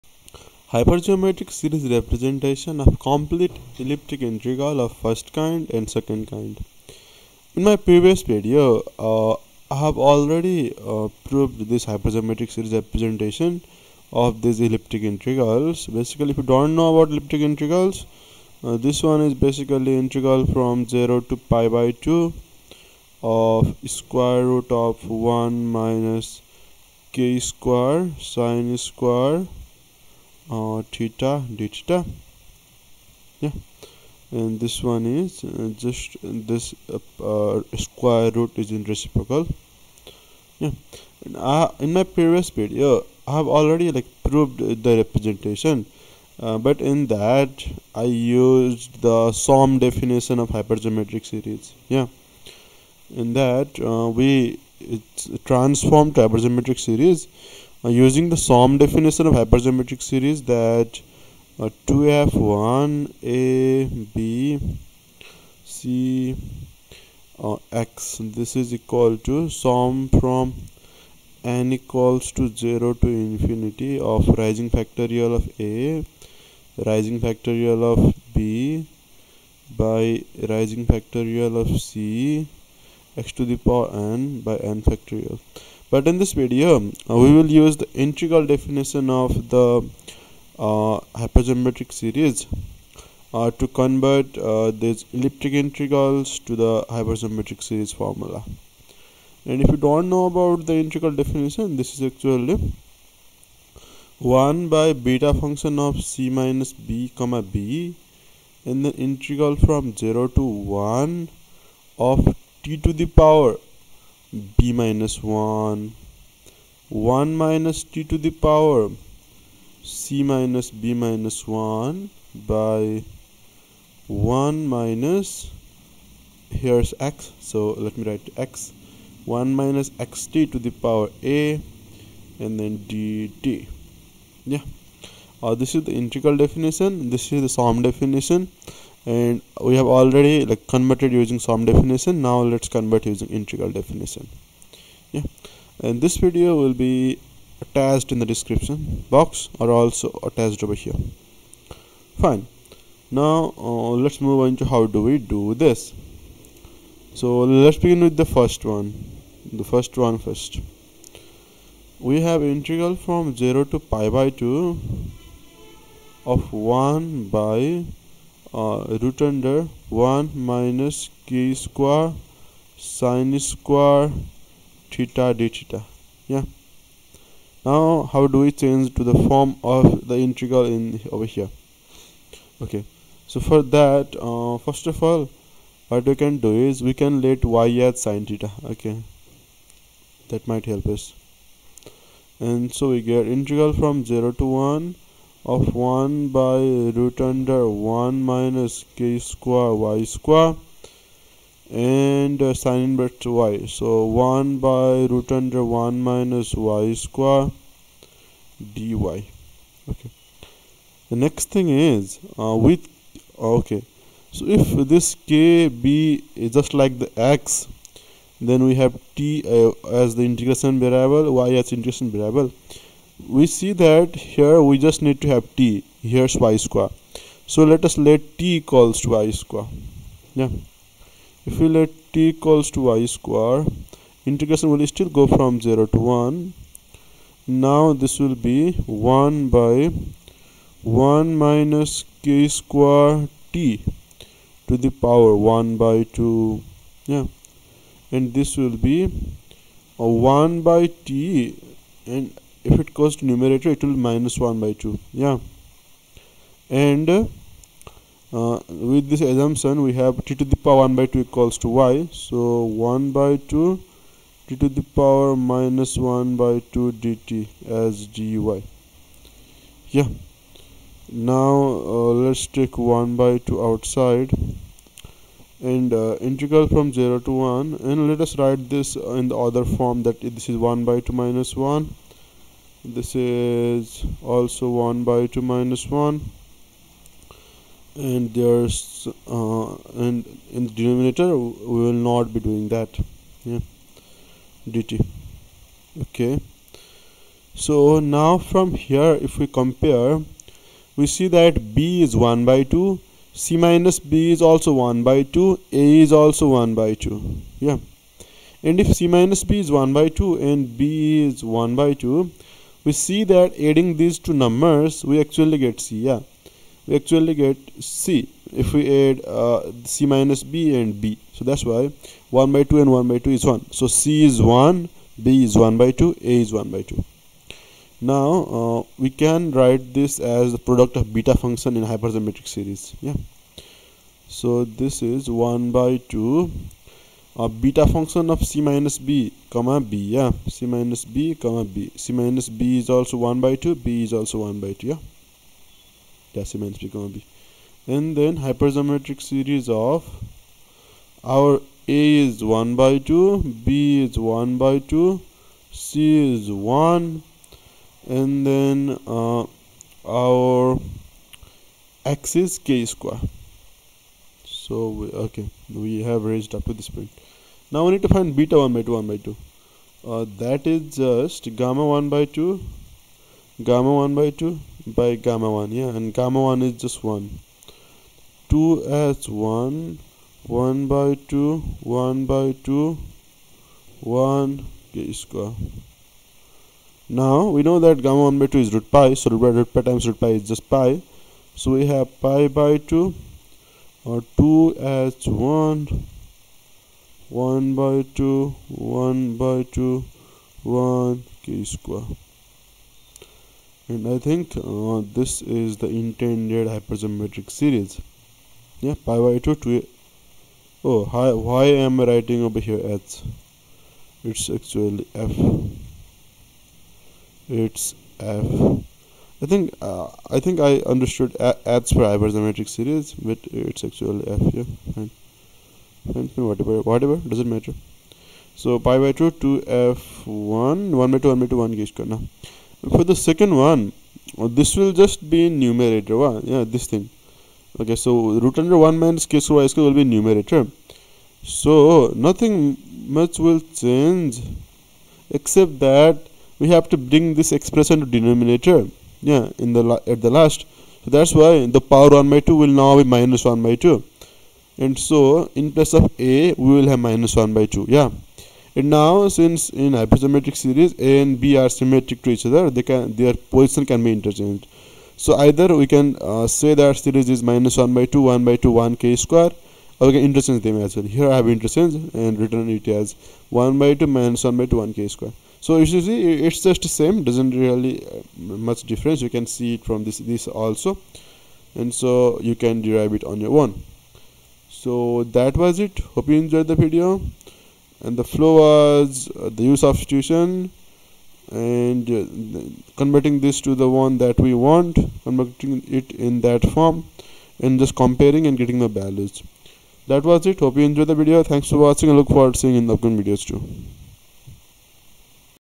Hyper Geometric Series Representation for Complete Elliptic Integral of First and Second Kind - II
We derive the hyper-geometric series representation for complete Elliptic Integral of first and second kind using integral definition of hyper-geometric series